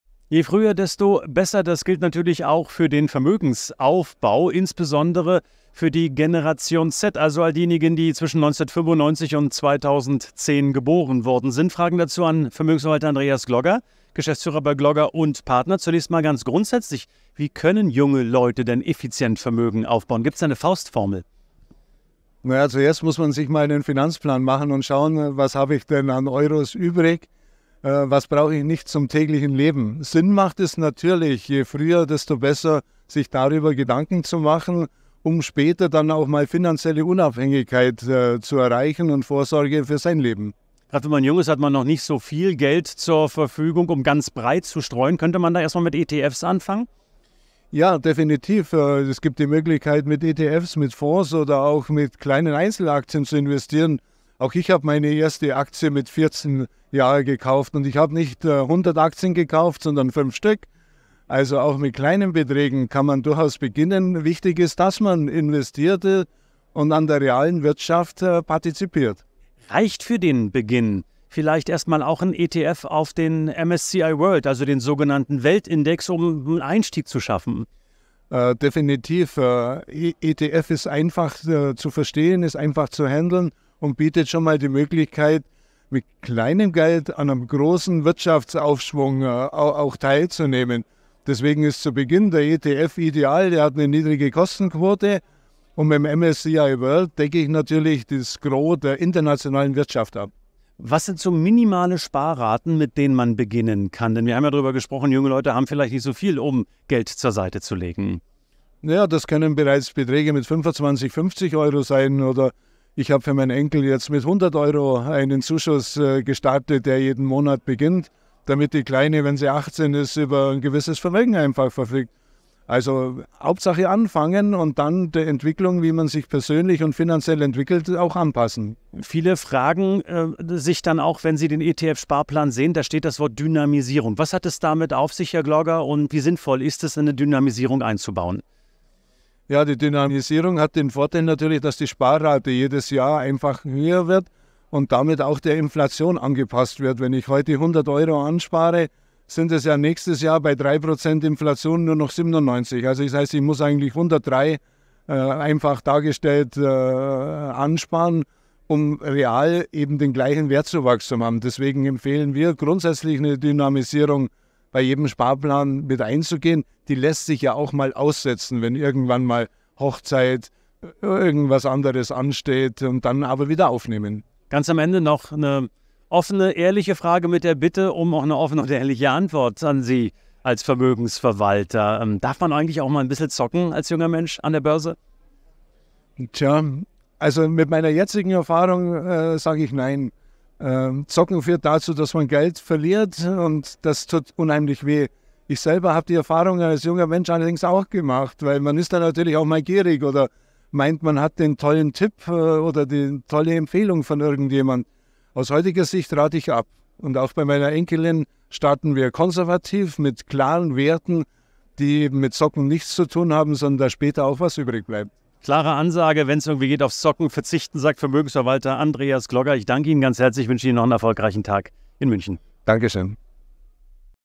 Aktien & ETFs für Generation Z: Wie können junge Leute am besten Vermögen aufbauen, Andreas Glogger?
Je früher, desto besser: das gilt auch für den Vermögensaufbau. Doch wie klappt das für die sogenannte Generation Z am besten? Welche Rolle spielen ETFs dabei und mit welchen Sparraten sollte man anfangen? Antworten von Vermögensverwalter Andreas Glogger, Geschäftsführer bei GLOGGER & PARTNER, im Interview mit Börsenmoderator Andreas Franik - aufgezeichnet auf dem Vermögenstag der V-Bank in München.